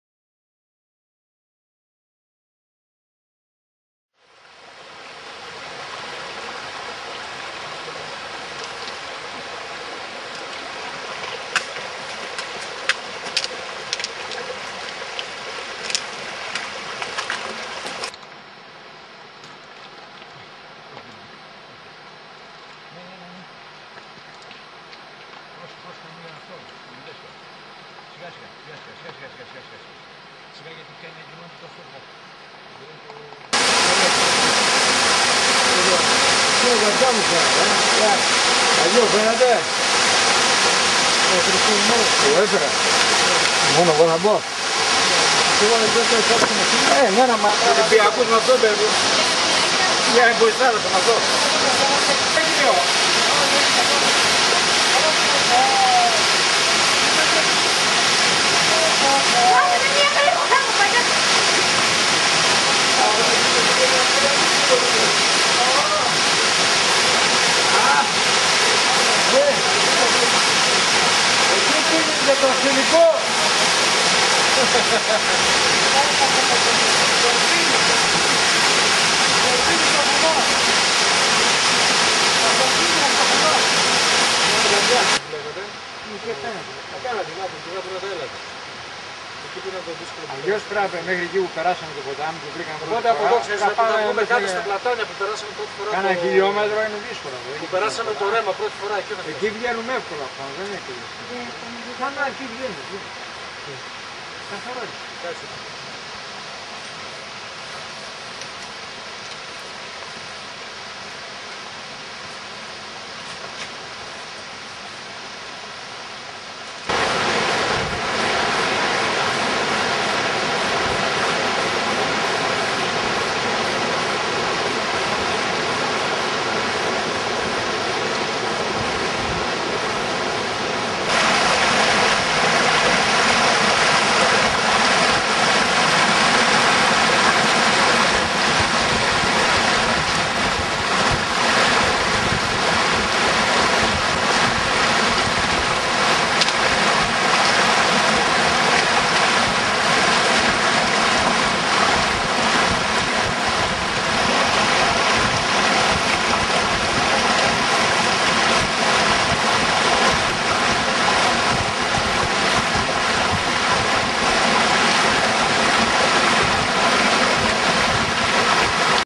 ΤΖΟΥΡΤΖΙΑ - ΚΑΤΑΡΡΑΚΤΗΣ "ΜΑΝΤΑΝΙ ΤΟΥ ΔΑΙΜΟΝΑ"
Η πορεία από την Τζούρτζια ως τον καταρράκτη έγινε από τα μέλη του Συλλόγου Πεζοπορίας - Ορειβασίας Τρικάλων στις 2/9/2012